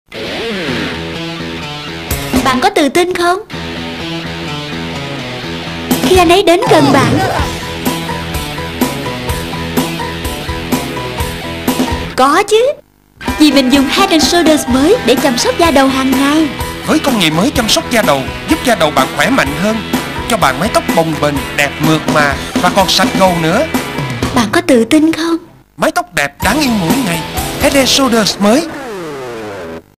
Quảng cáo Head & Shoulders - Tự tin đến gần (2000) [30s]
Clip quảng cáo Dầu gội trị gàu Head & Shoulders 💙 (với Công nghệ mới Chăm sóc da đầu).